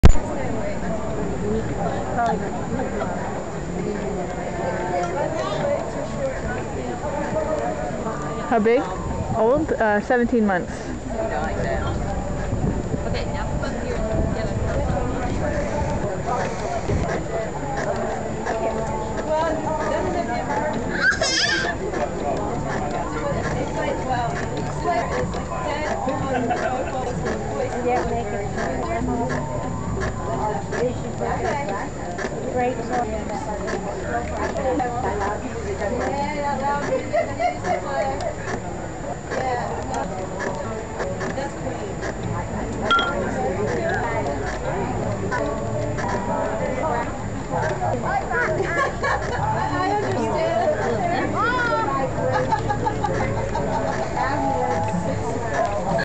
Treadle pottery wheel
eastknollpottery.com making miniature pots at the Mercer Museums Folk Fest 2009.  Isobel "helping".